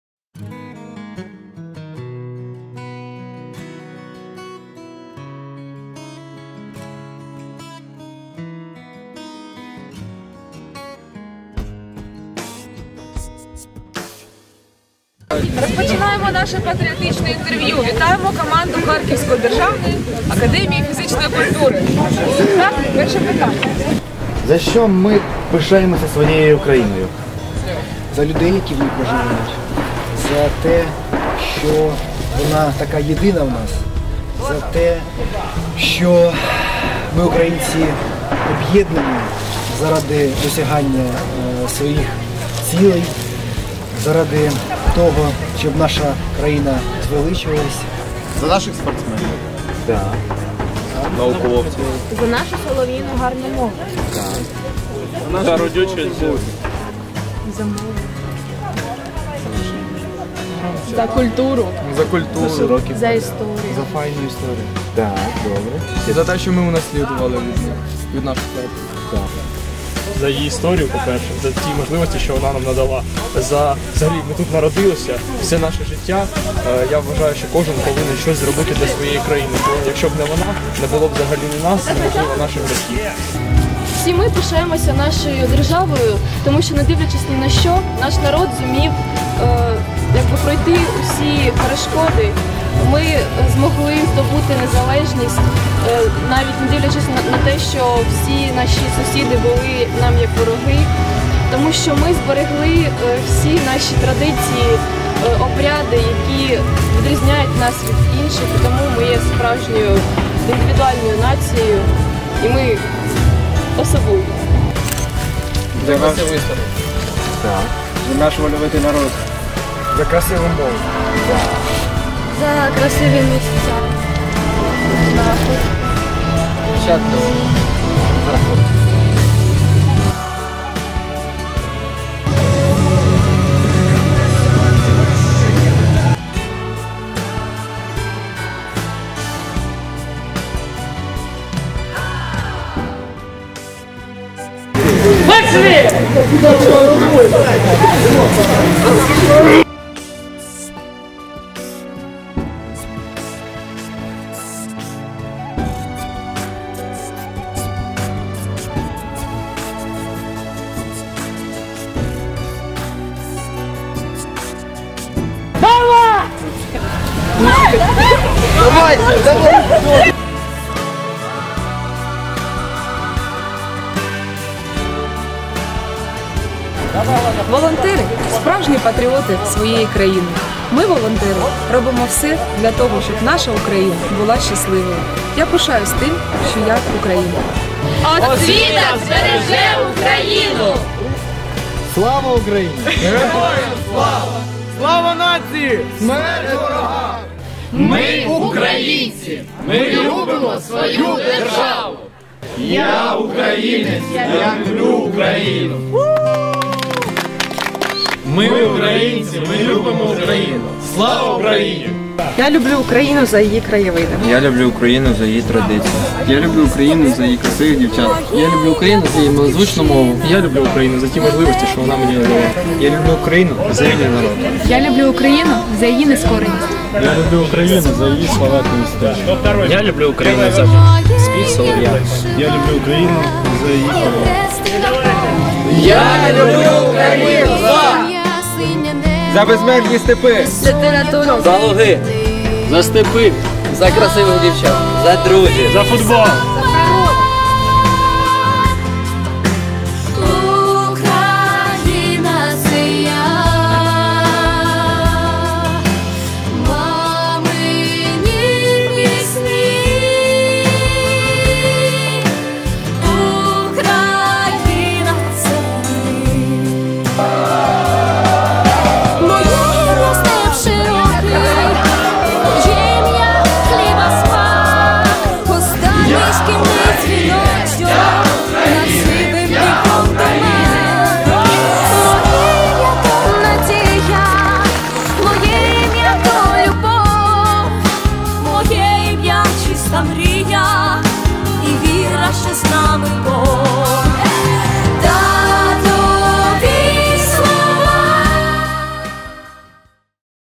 Обласний табір (гра-вишкіл) Я - Українець! 3
Світлана Сітнік - президент Харківської обласної молодіжної громадської організації "Харківський центр волонтерів"

Название мероприятия:

Обласний табір (гра-вишкіл), спрямований на збільшення чисельності молоді, яка пишається своїм українським походженням "Я - Українець!"

за підтримки управління у справах молоді та спорту Харківської обласної державної адміністрації

Громадська організація «Адаптаційний центр», «Слобідське Козацьке Військо»
Харківська обласна молодіжна громадська організація «Харківський  центр волонтерів» за ініціативи членів Координаційної ради з питань національно-патріотичного виховання при Харківській обласній державній адміністрації
Обласний табір (гра-вишкіл) «Я-Українець!»
 Уважаемый …
Если Вам интересны другие материалы нашего производства, то позвольте рекомендовать Вам